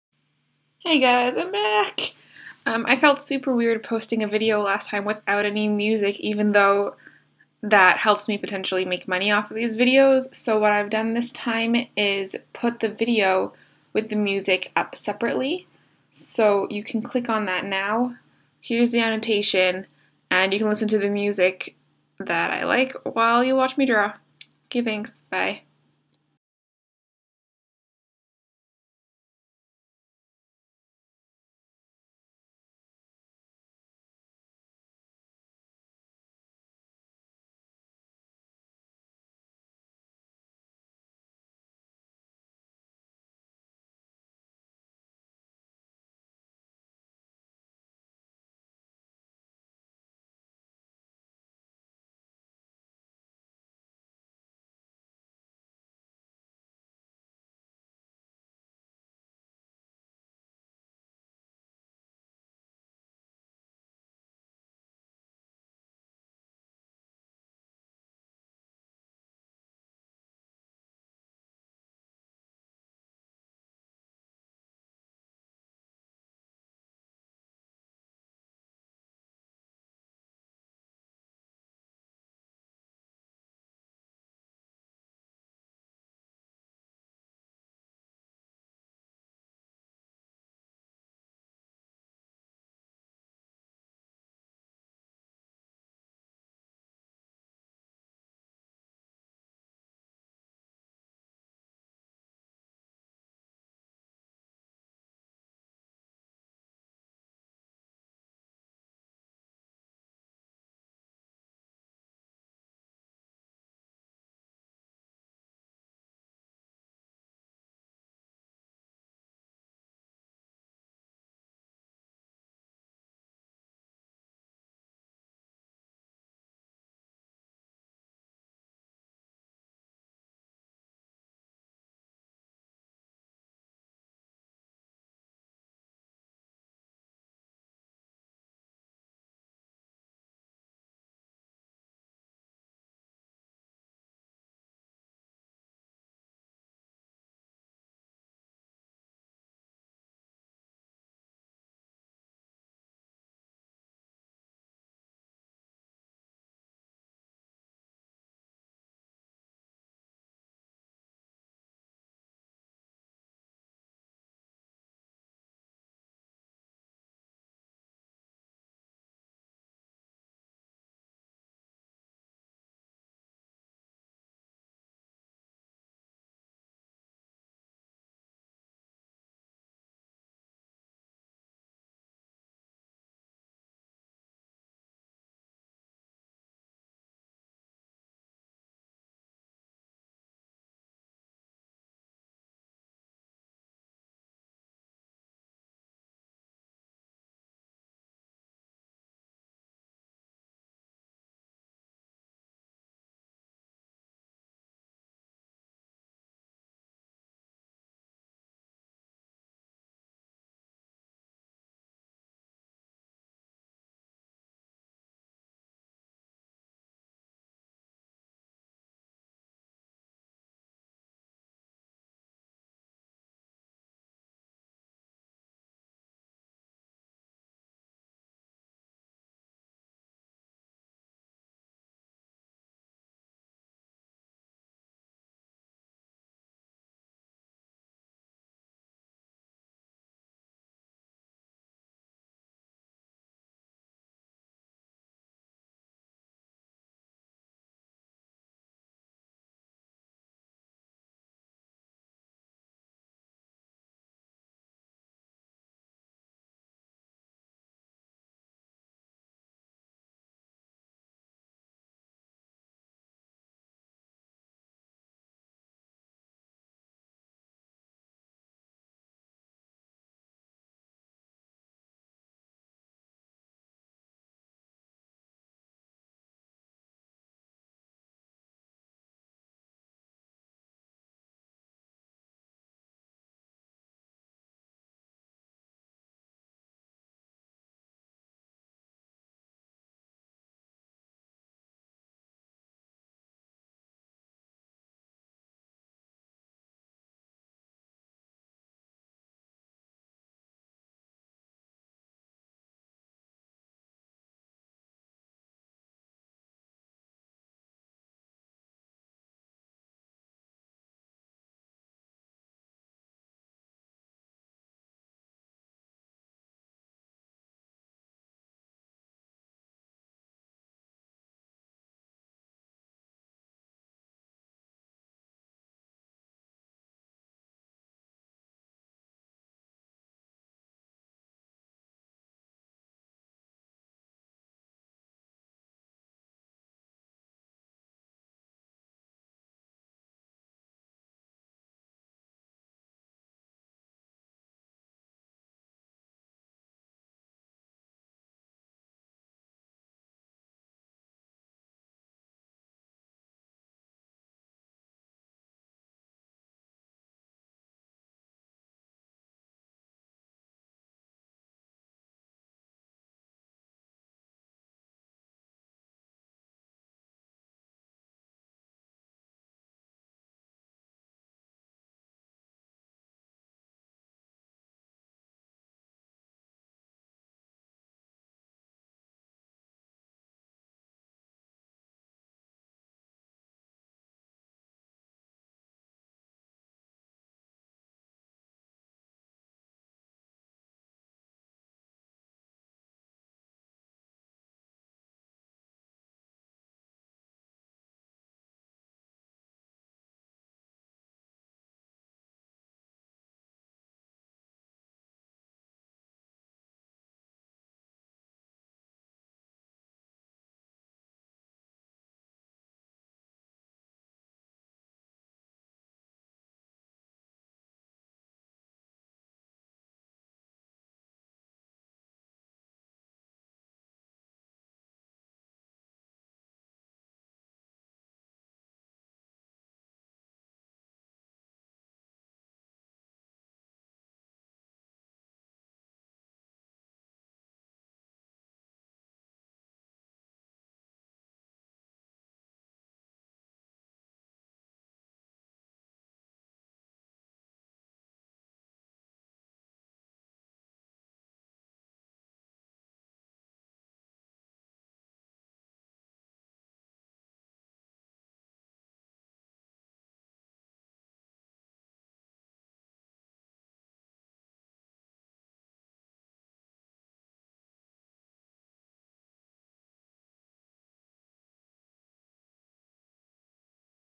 Drawing a Random Fancy Lady
used adobe illustrator; all pen/pencil tool! no meshes!